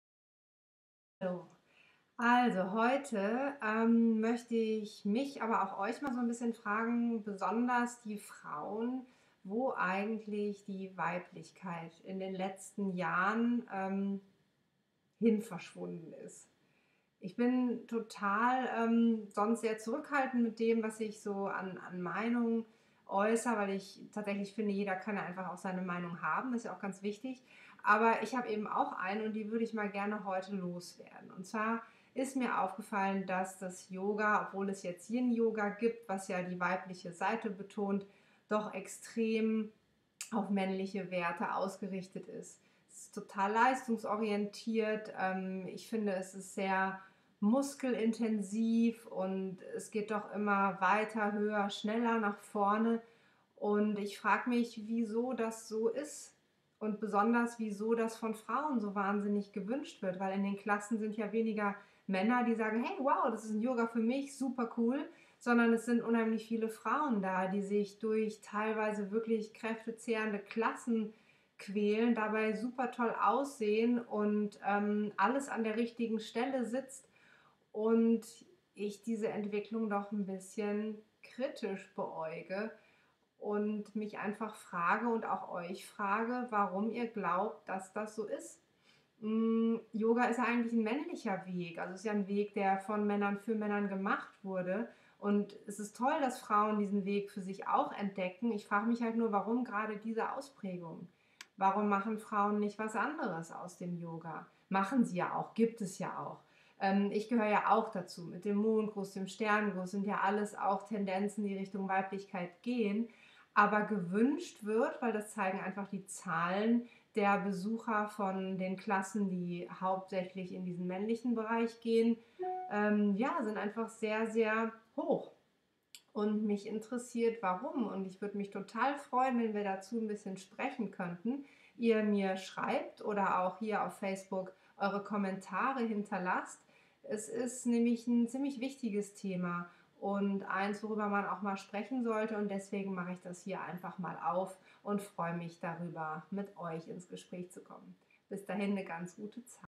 Futsch? Die Weiblichkeit im Yoga.
Katharina Middendorf spricht über Weiblichkeit im Yoga und wie sie dazu steht.